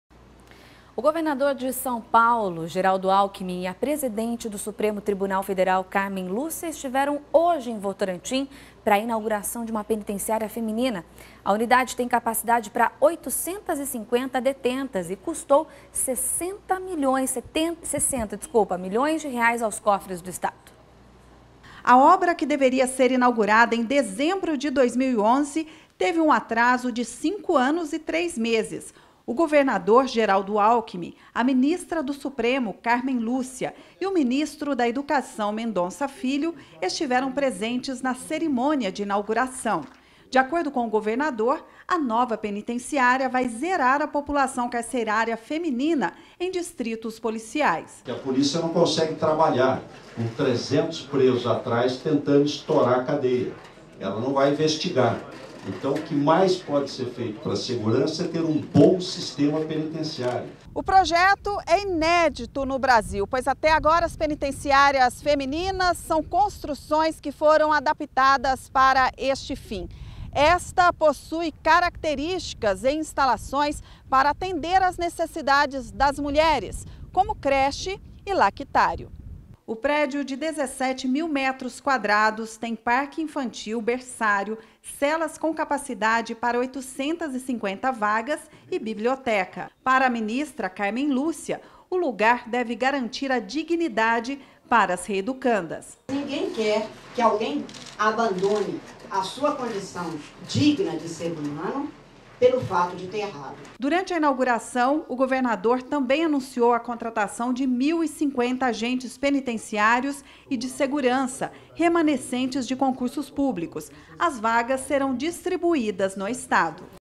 Votorantim ganha penitenciária
O Governador de São Paulo, Geraldo Alckmin, e a Presidente do Supremo Tribunal Federal, Cármen Lúcia, estiveram hoje em Votorantim, para a inauguração de uma penitenciária feminina. A unidade tem capacidade para 850 detentas e custou R$ 60 milhões aos cofres do estado.